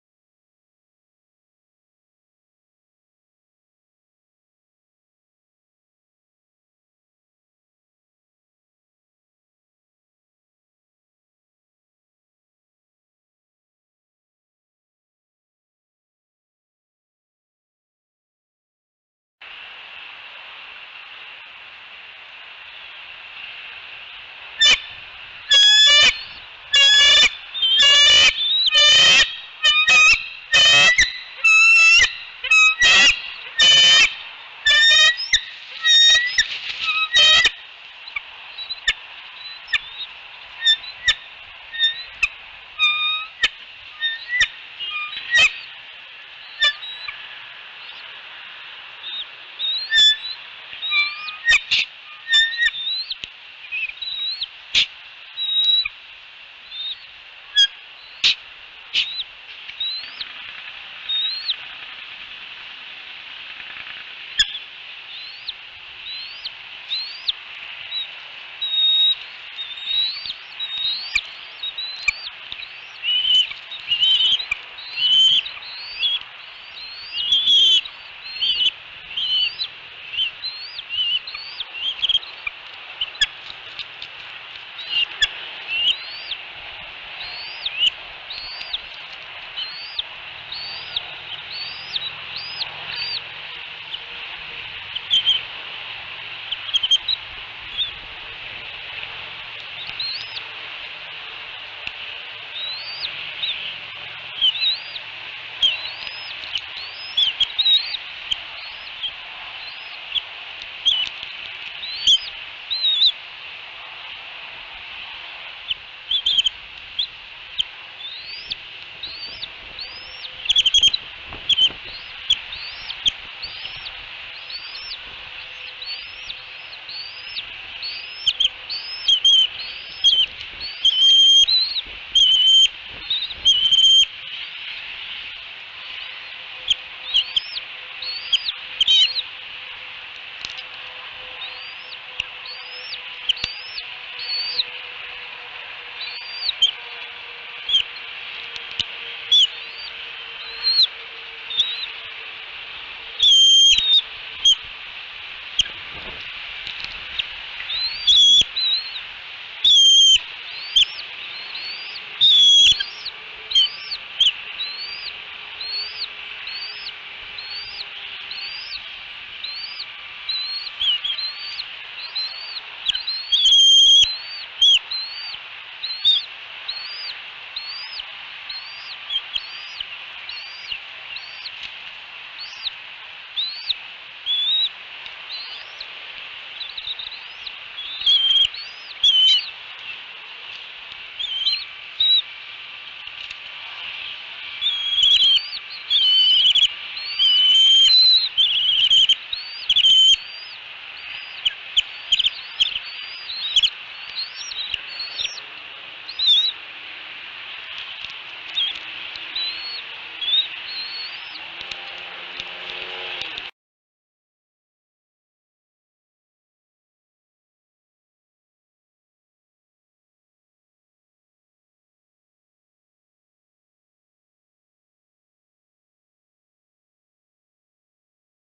AEF SM Eagle Cam 4-18-18: Two Fishes!
In the Smoky Mountain Nest of Bald Eagles Lady Independence & Sir Hatcher II, there is always a lot of food in the pantry. In this video by lil_AEF, we see Lady with a fish  and Sir Hatcher flying in with yet another one!  One of the eaglets (it's the smallest, so I think SM16)  turns around to begin eating Dad's fish with gusto!